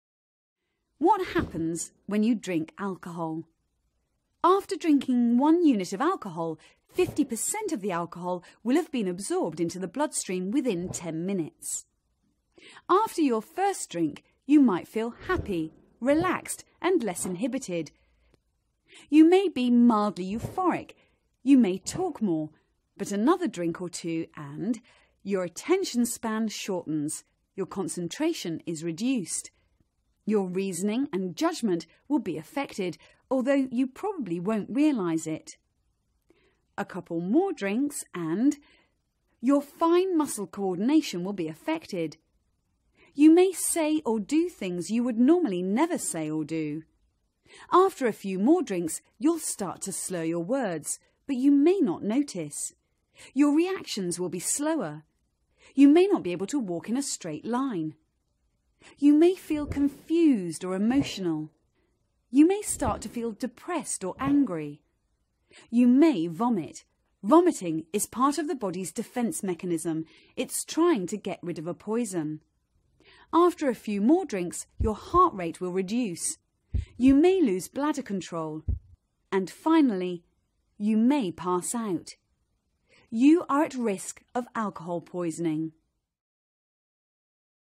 The Effects of Alcohol | iHASCO
What happens when you drink alcohol?

After drinking 1 unit of alcohol, 50% of the alcohol will have been absorbed into the bloodstream within 10 minutes.
After your first drink you might feel happy, relaxed and less inhibited
You may be mildly euphoric, you may talk more, but another drink or two and...
Your attention span shortens, your concentration is reduced.
Your reasoning and judgement will be affected, although you probably won’t realise it. A couple more drinks and...
Your fine muscle coordination will be affected.
You may say or do things you would normally never say or do.
After a few more drinks you’ll start to slur your words, but you may not notice.
Your reactions will be slower.
You may not be able to walk in a straight line.
You may feel confused or emotional.
You may start to feel depressed or angry.
You may vomit. Vomiting is part of the body’s defence mechanism – it’s trying to get rid of a poison.
After a few more drinks your heart rate will reduce.
You may lose bladder control; and finally...
You may pass out. You are at risk of alcohol poisoning.